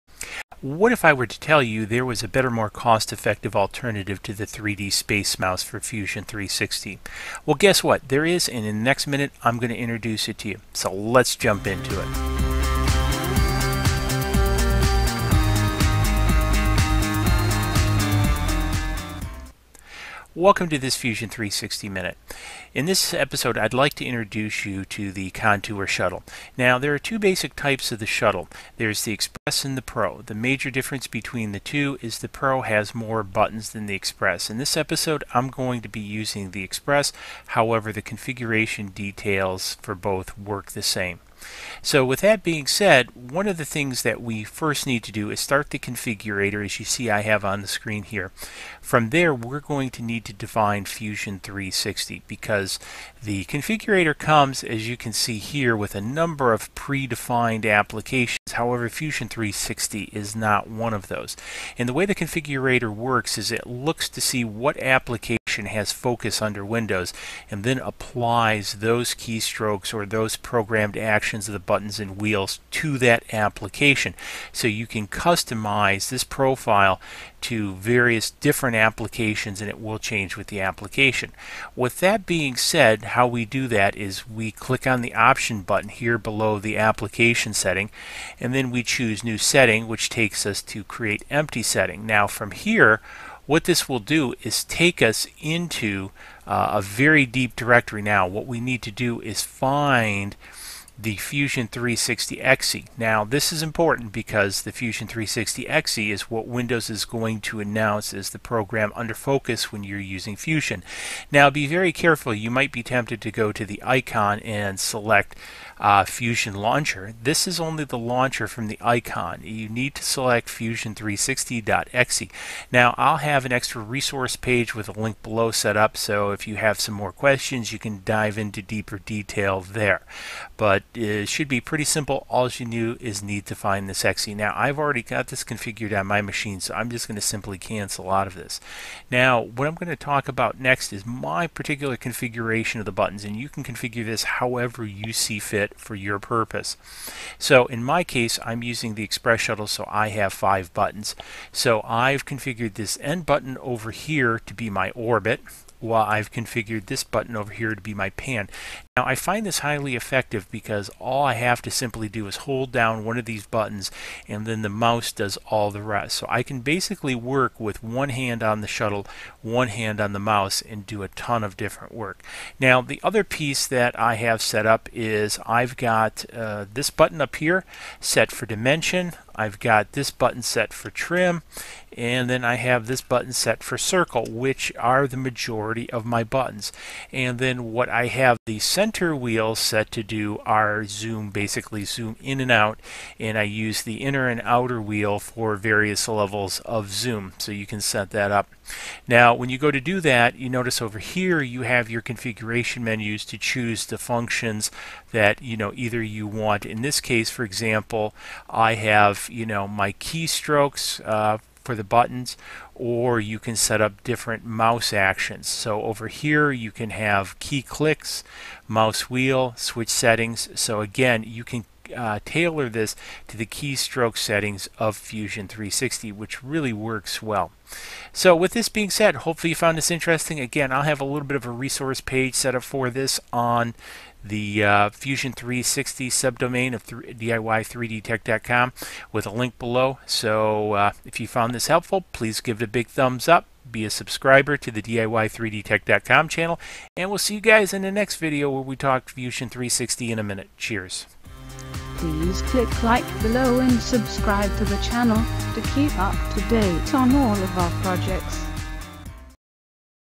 Fusion 360 Minute - Configuring the Contour Shuttle!
In this episode of Fusion 360 Minute, we will be looking at how to configure a more cost effective alternative to the space mouse (see more ) using either the Contour Shuttle Express (see more ) or the Contour Shuttle Pro (see more  As being able to work two handed with Fusion 360 has greatly enhanced our productivity!

If you want more Fusion 360 Minute, check out our Fusion 360 playlist

to what we are up too as we always have something cooking in the shop!